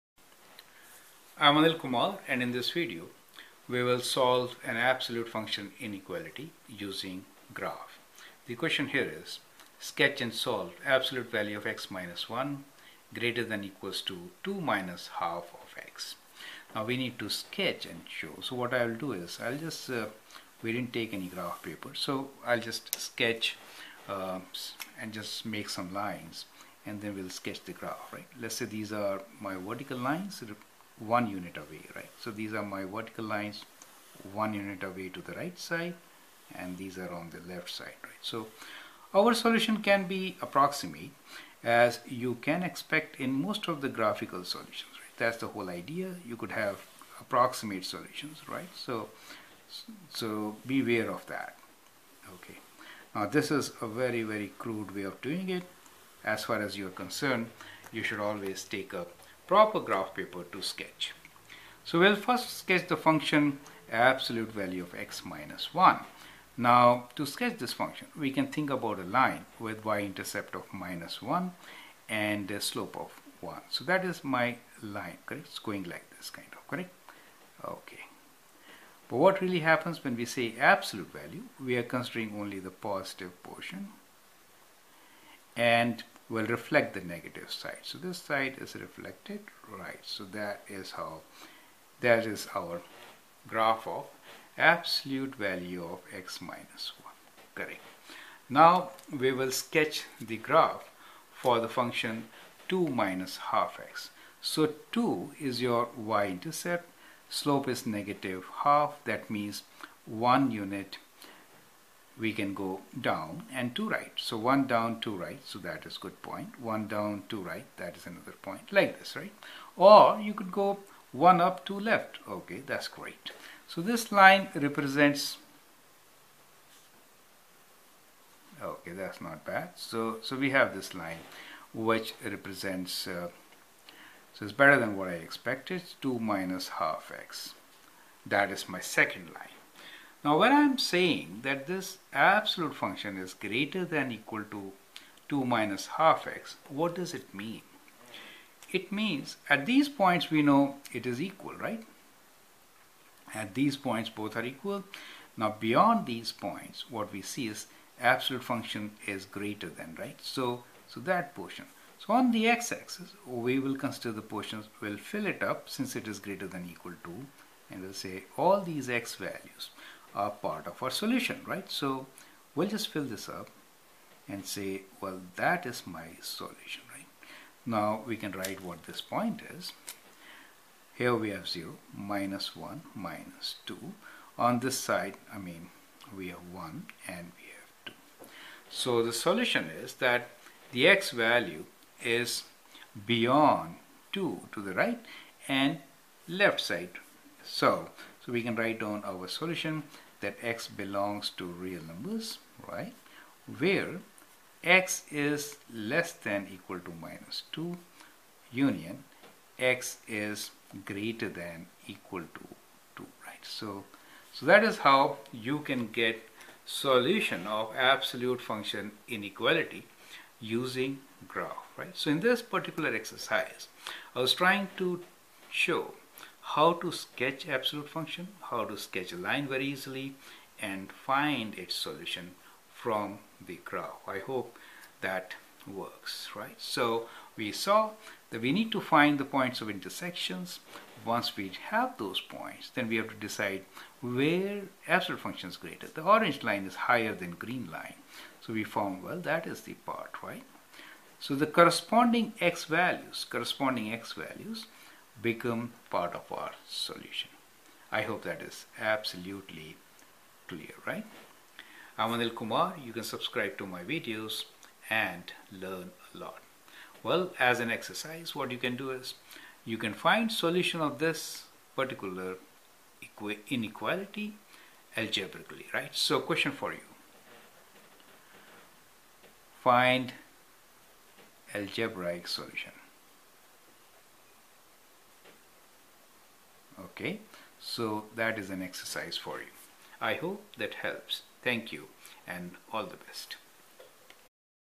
Solve Absolute Function Inequality Using Graph Greater than Line
Solve the following equations
a. |x-3|=7  b. |2x+5|=17
c. |-2x-6|=0  d. |x-5|=-1
e. |8-2x|=6  f. |x-2|=|2x-1|
g. |2-3x|=|x-4|  h. |x^3-14|=13